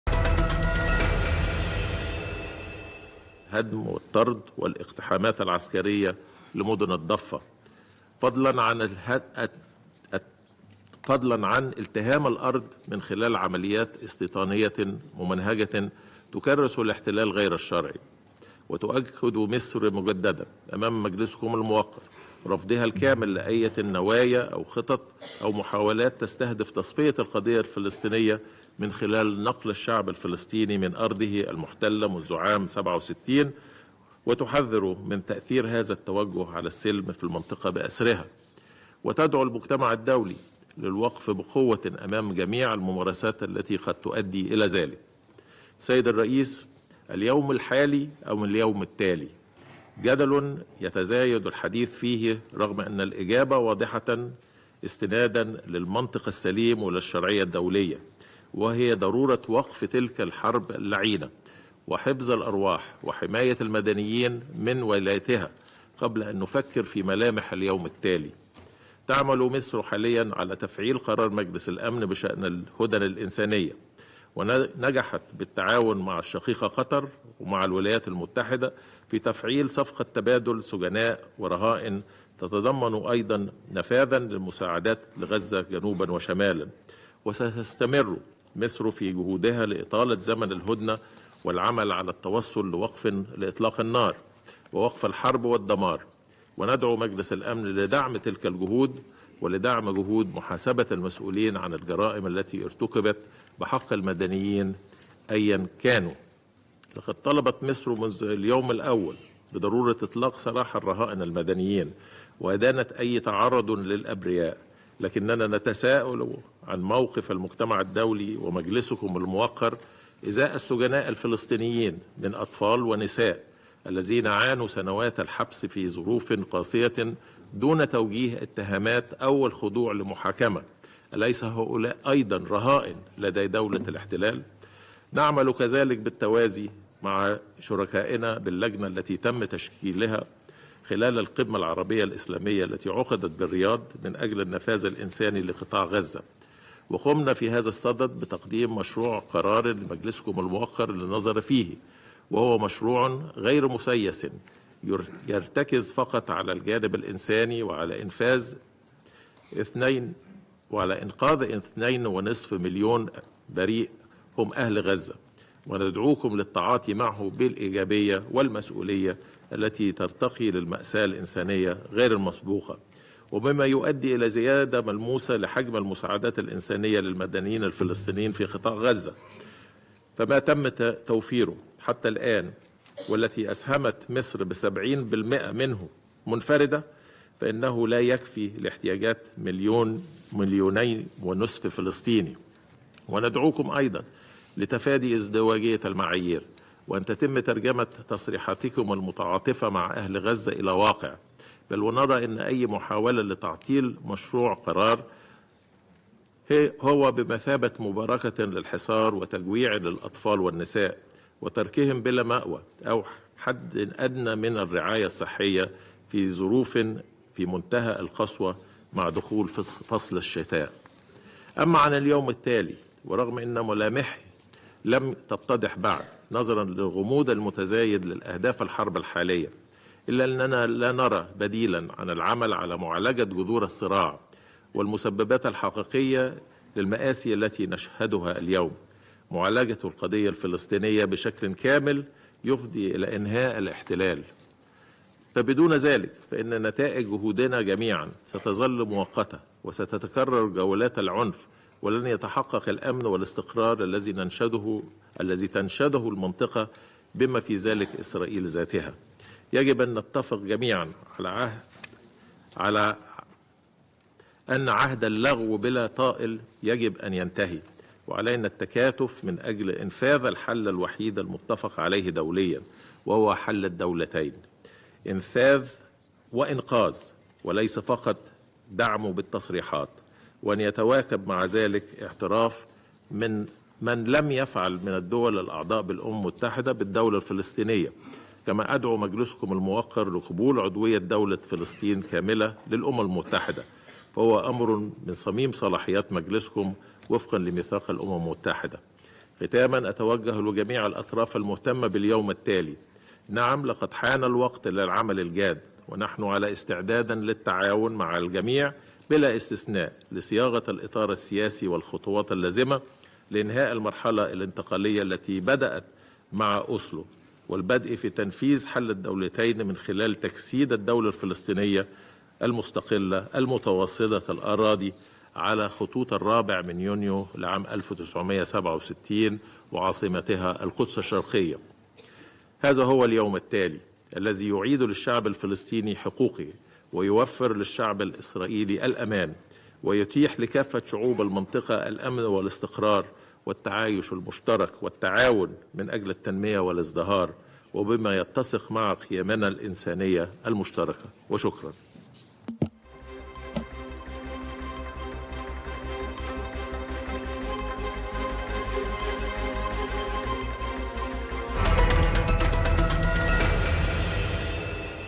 كلمة وزير الخارجية سامح شكري خلال جلسة مجلس الأمن الدولي بشأن القضية الفلـ ـسطينية
تغطية خاصة | كلمة وزير الخارجية سامح شكري خلال جلسة مجلس الأمن الدولي بشأن القضية الفلـ ـسطينية